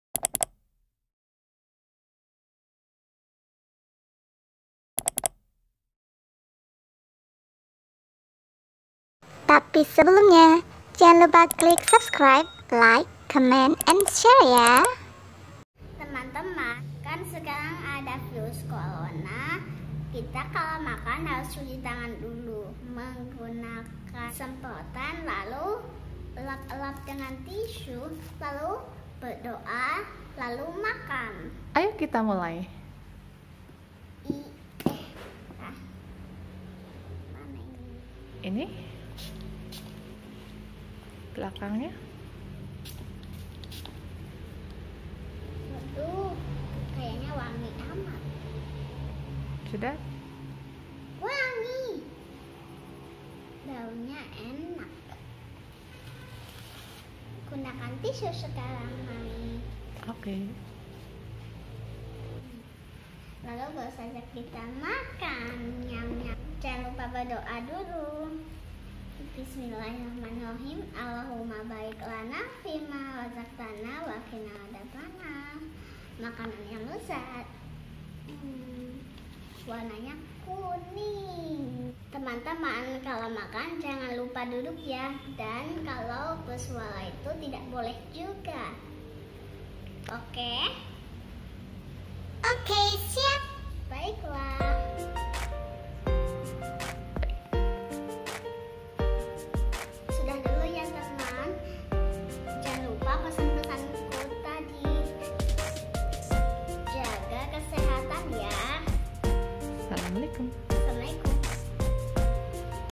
Bianca Belajar Mengaji (BBM) - Doa Sebelum Makan
Assalamu'alaikum. Bianca kembali lagi, Teman. Yuk, belajar mengaji! Kali ini kita belajar doa sebelum makan dan apa saja yang dilakukan sebelum makan. Thank you 🙏 Mohon maaf jika ada kesalahan/kekurangan.
▶️ Ms. Lintang Kusuma
▶️ Bianca Rashaad